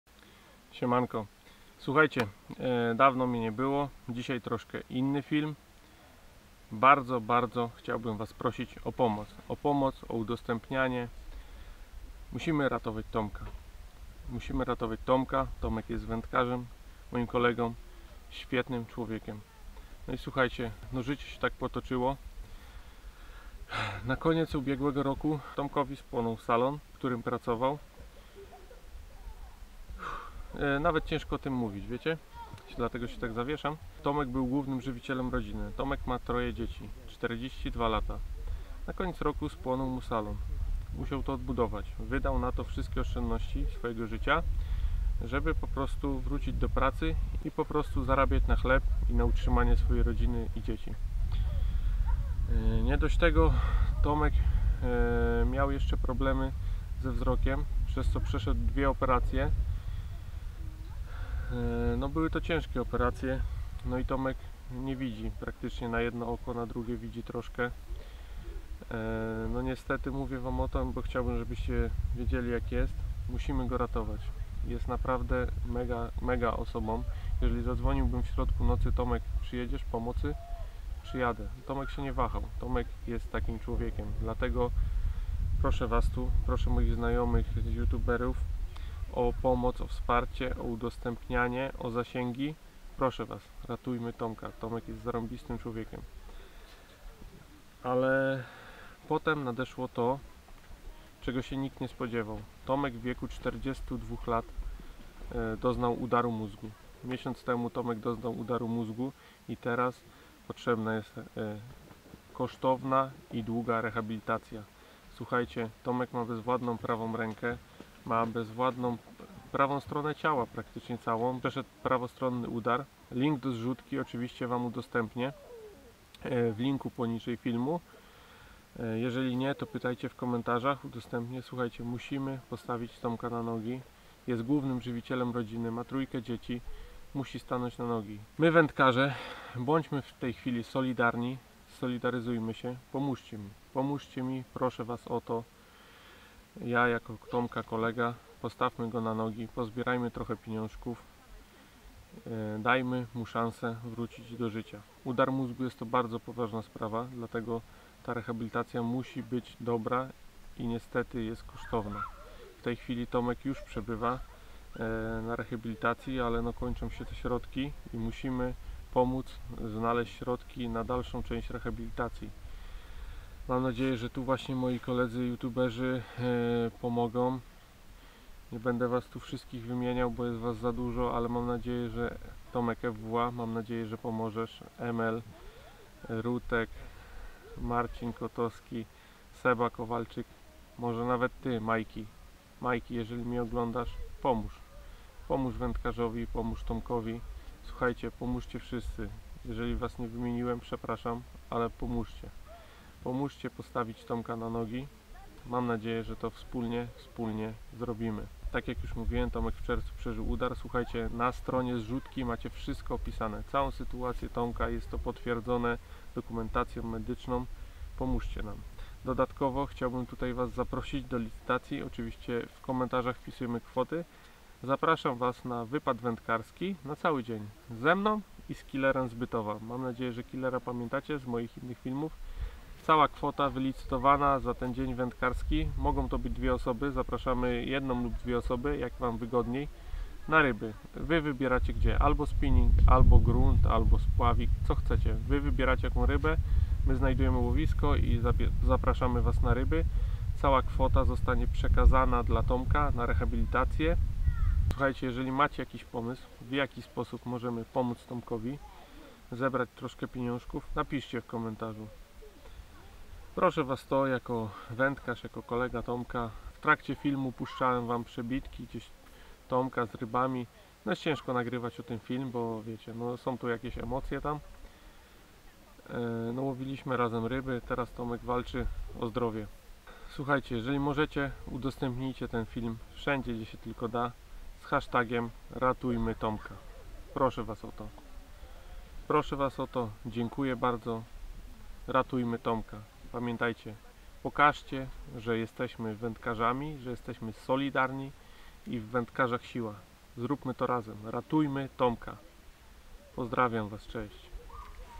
POMOC dla TOMKA !! Wędkarz potrzebuje naszej pomocy czyli rehabilitacja po udarze
Proszę WAS o pomoc i ratowanie TOMKA
UDOSTĘPNIJ FILM , daj łapkę i komentarz dla zasięgu

DZIĘKUJE !